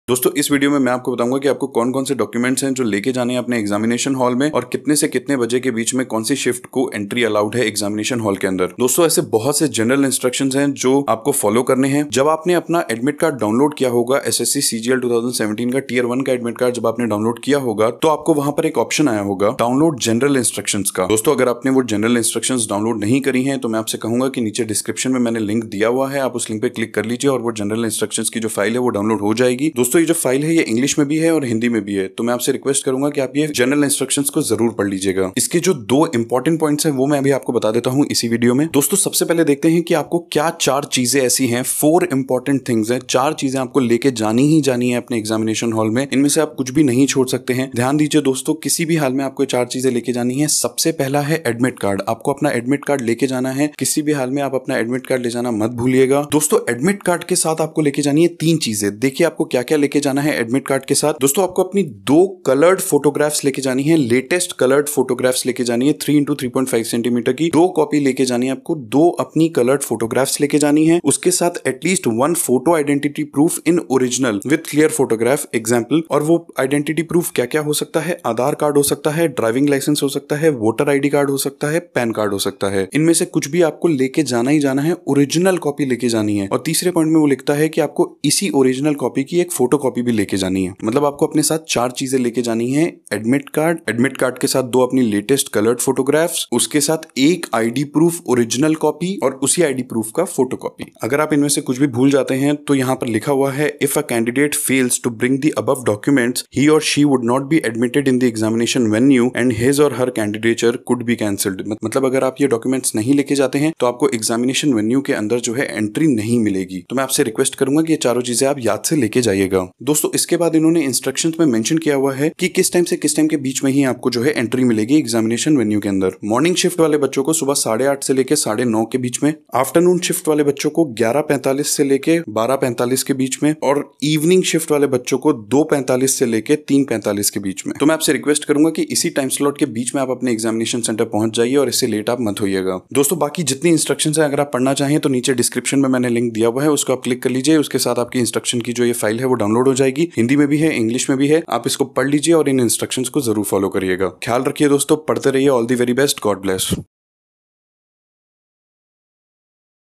DOCUMENTS TO CARRY FOR SSC CGL 2017 TIER 1 EXAM IN EXAMINATION HALL AND ENTRY TIMINGS
Must watch this video to know what documents you have to take along with your admit card and all the general instructions that you need to follow for your ssc cgl 2017 tier 1 exam.